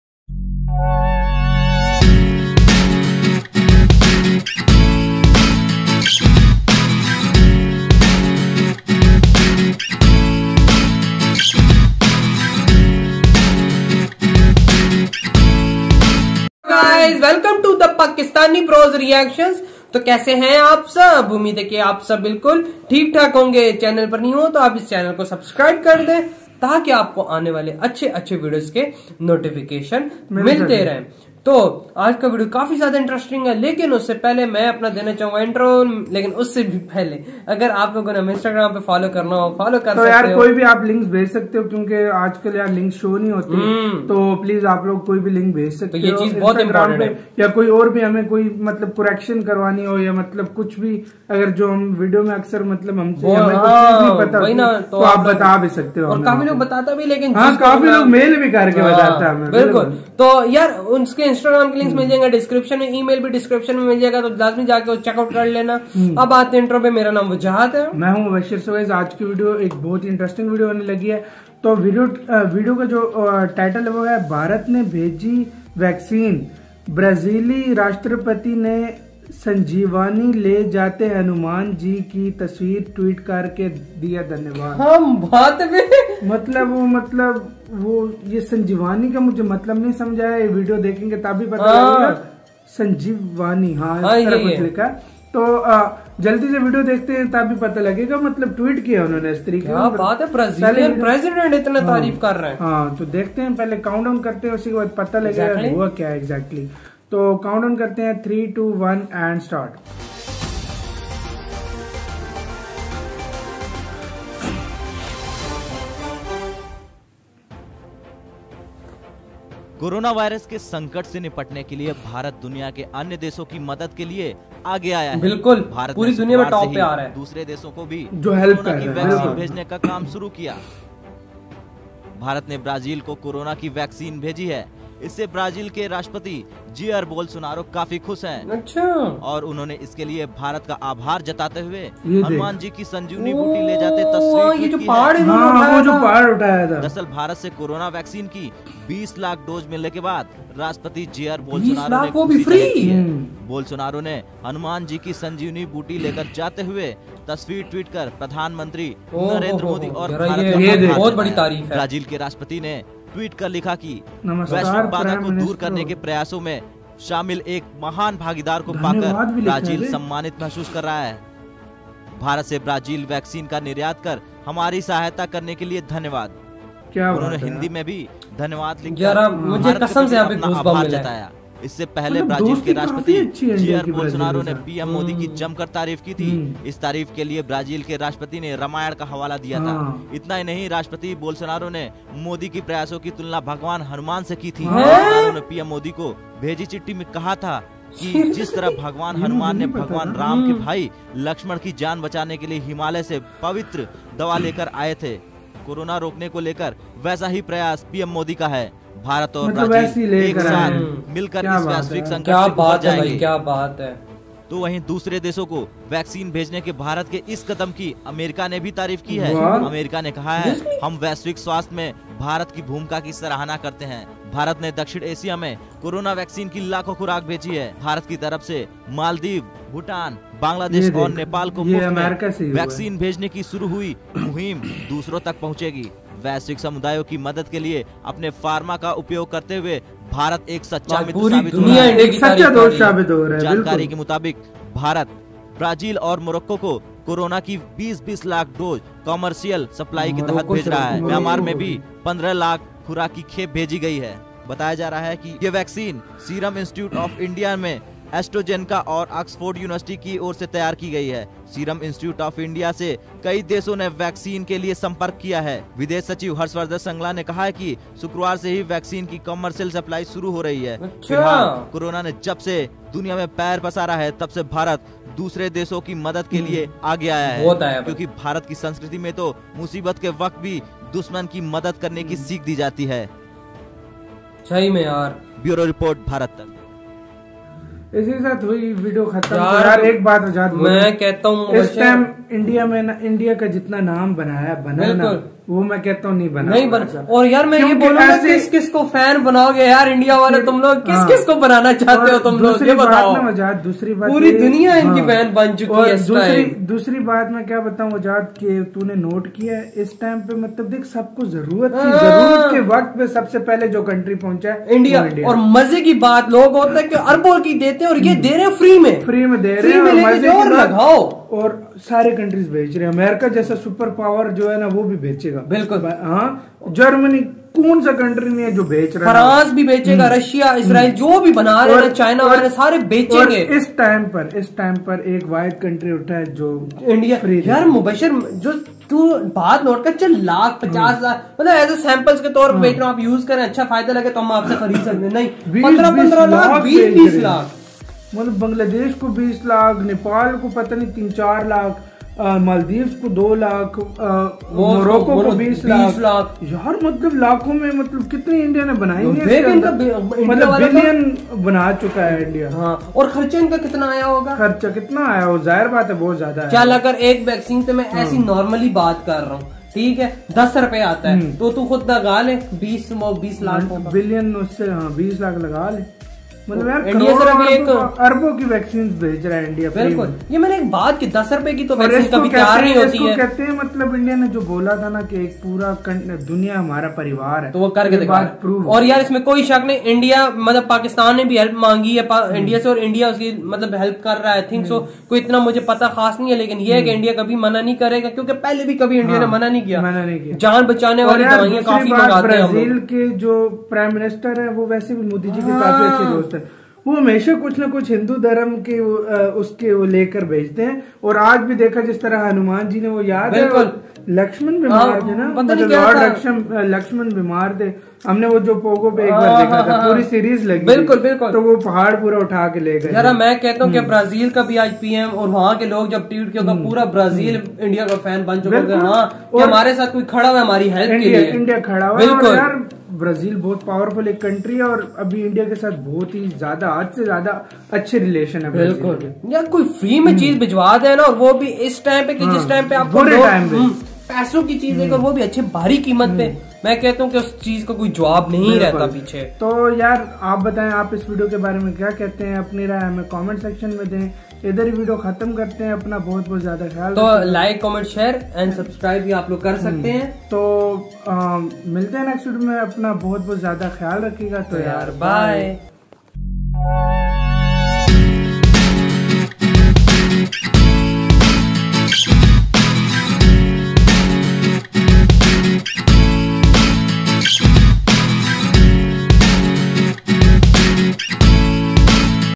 भारत ने भेजी वैक्सीन,ब्राजीली राष्ट्रपति ने संजीवनी ले जाते हनुमान जी की तस्वीर ट्वीट कर कहा धन्यवाद
OUR BACKUP CHENNAL IS:-

.credits.

     Mubashir:- Mubashirktk007
     Wajahat:-    Sheikhwajahat007